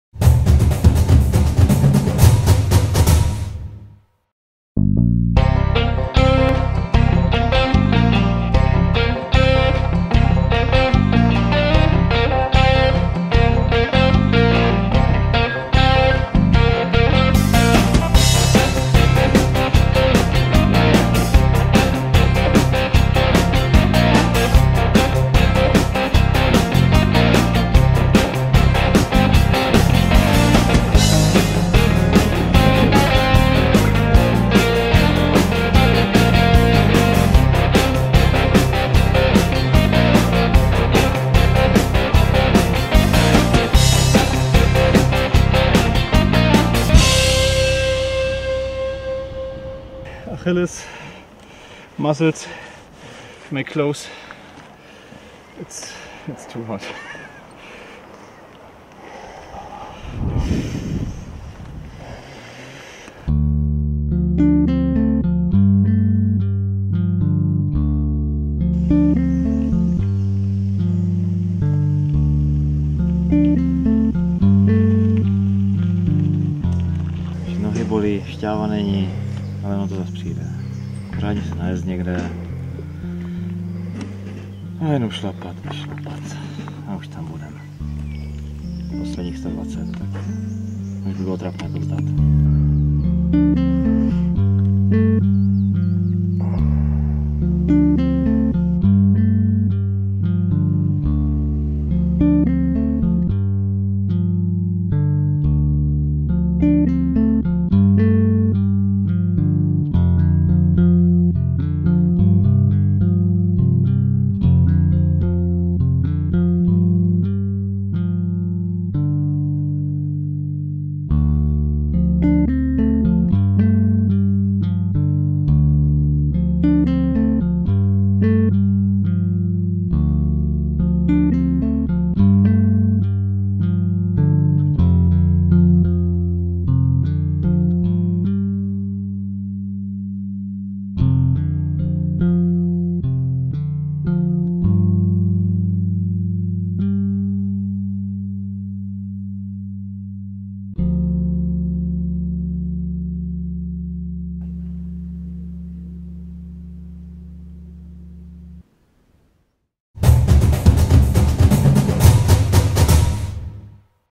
Krušné hory 2017
Poslední pohoří, které musí závodníci ISOMA 1000 Miles Adventure překonat, jsou Krušné hory. Za nimi je čeká vytoužený cíl ve městě Skalná.